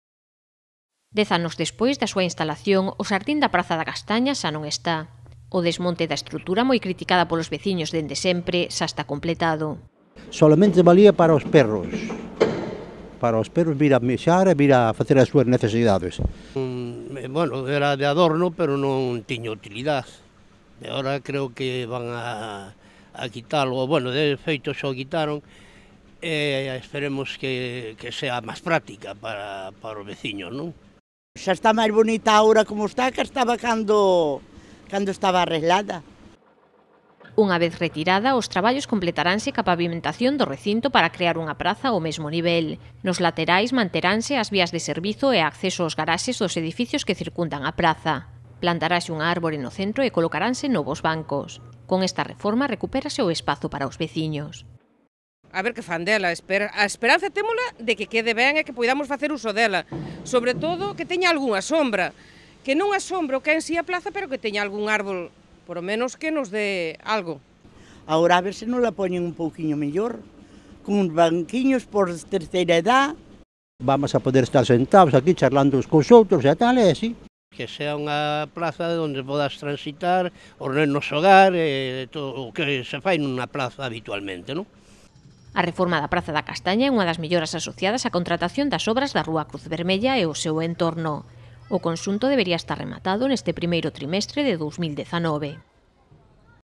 A praza da Castaña di adeus ó seu xardín elevado
PONTEVEDRA. A praza da Castaña terá un novo aspecto moi pronto. Como parte da reforma da rúa Cruz Vermella e o seu entorno, desmontouse o xardín elevado que ocupaba case todo o espazo e que dende a súa instalación provocou o rexeitamento dos veciños.

Dez anos despois da súa instalación, o xardín da Praza da Castaña xa non está. O desmonte da estrutura, moi criticada polos veciños dende sempre, xa está completado.

Unha vez retirada, os traballos completaranse coa pavimentación do recinto para crear unha praza ó mesmo nivel. Nos laterais manteranse as vías de servizo e acceso ós garaxes dos edificios que circundan a praza. Plantarase unha árbore no centro e colocaranse novos bancos. Con esta reforma recupérase o espazo para os veciños.

A reforma da praza da Castaña é unha das melloras asociadas á contratación das obras da rúa Cruz Vermella e o seu entorno. O conxunto debería estar rematado neste primeiro trimestre de 2019.